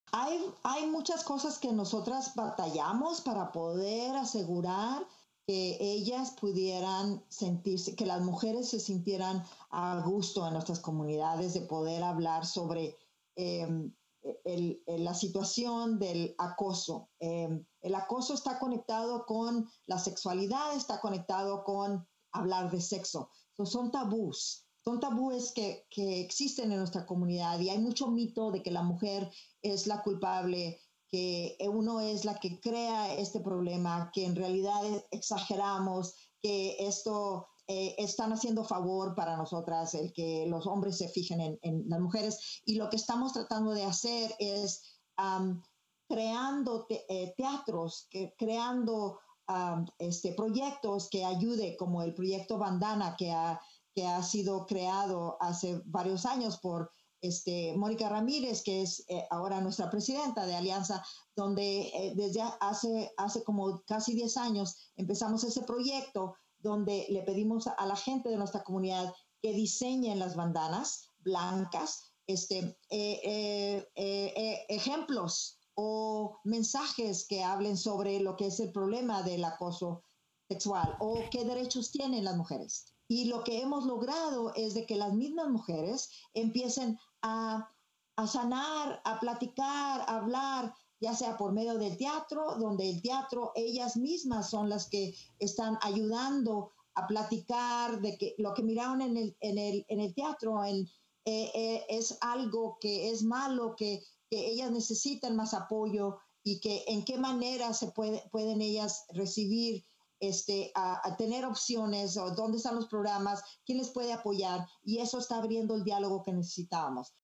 Trabajadoras de sectores de bajos salarios enfrentan serios problemas al denunciar el acoso sexual
Vea esta y otras noticias en nuestra página web en español

Analizamos cómo el abuso sexual es también algo común en los sectores de bajos salarios, como la industria agrícola, los servicios de limpieza de hoteles, o el sector doméstico, donde los puestos de trabajo son ocupados de forma desproporcionada por mujeres de color y mujeres inmigrantes, quienes son extremadamente vulnerables a al acoso y la violencia sexual.